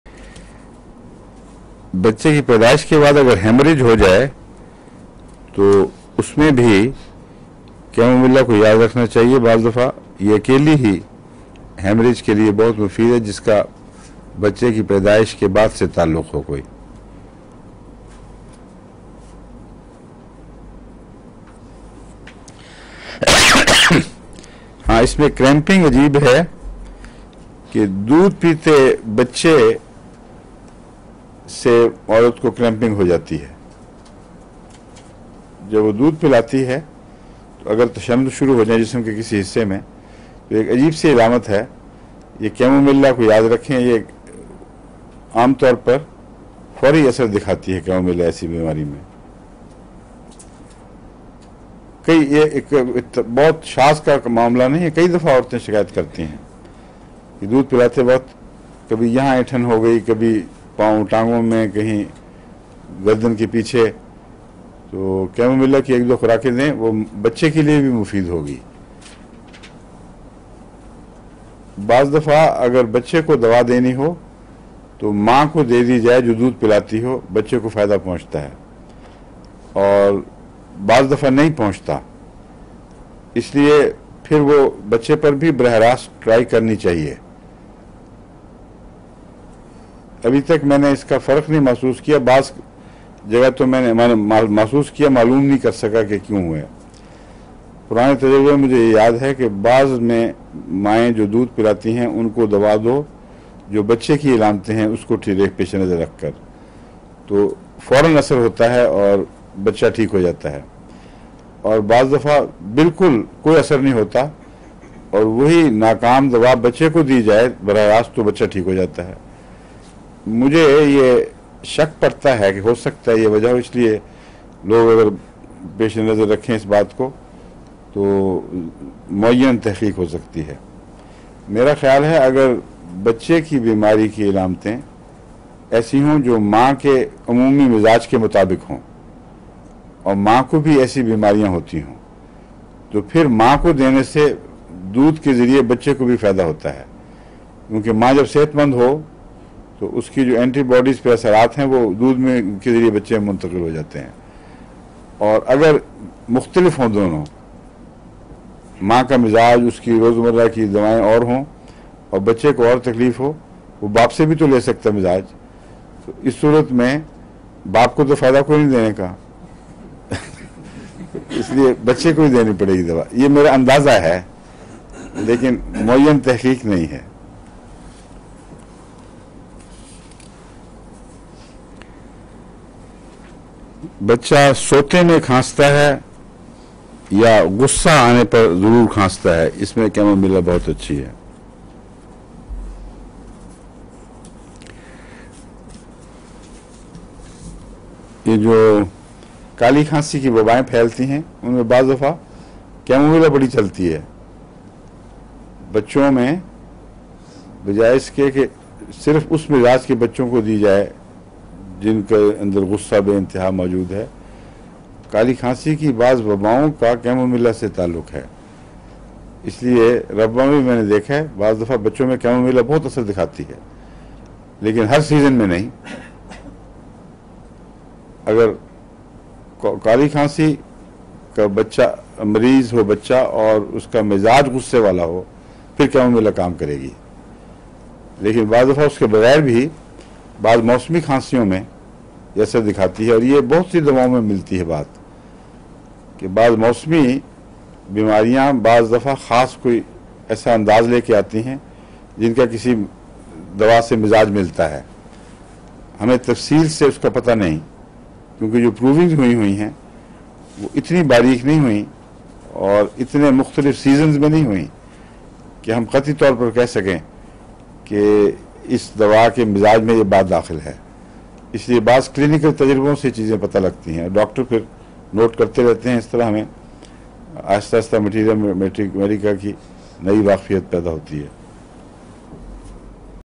Medicine for breastfeeding mothers and infants by Hazrat Mirza Tahir Ahmad
What medications are safe to take while breast-feeding?
Acetaminophen (Tylenol, others)
Ibuprofen (Advil, Motrin IB, others)
Naproxen (Naprosyn) — short-term use only.
Medicines and breastfeeding safety

Most medicines are safe to take while you are breastfeeding because they do not pass into your breast milk. Even if the medicine does enter your milk, it is usually in such a small amount that it will not affect your baby.
Is paracetamol safe for breastfeeding?
Paracetamol is the first choice of painkiller if you are breastfeeding. It appears in breast milk in very small amounts which are very unlikely to harm your baby. If you take paracetamol when breastfeeding, make sure you take it at the recommended dose and for the shortest possible time.
Welcome to my homeopathic treatment channel, where you will be guided on the diagnosis, symptoms and treatment of diseases.